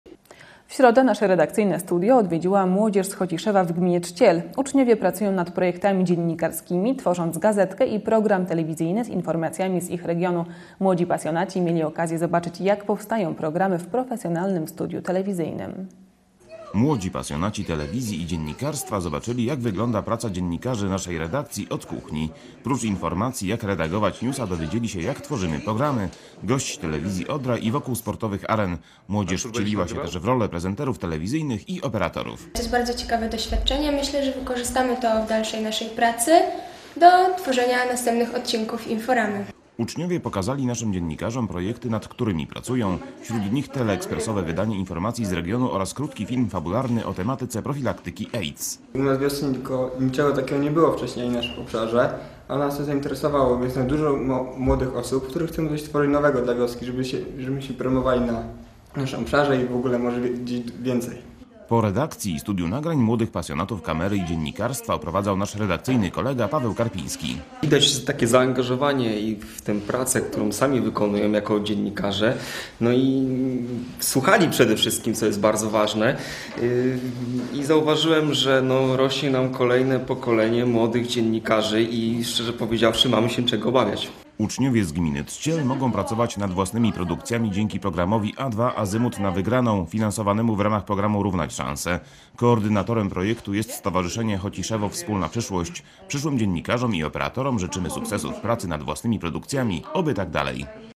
Redakcja Azymut TV w redakcji Odra TV
Grupa młodzieży uczestniczącej w projekcie "A2 - Azymut na wygraną" odwiedziła telewizję "Odra". Dziennikarze pokazali profesjonalne studio telewizyjne, sprzęt oraz to jak wygląda praca w telewizji.

Fragment wiadomości z telewizji ODRA TV.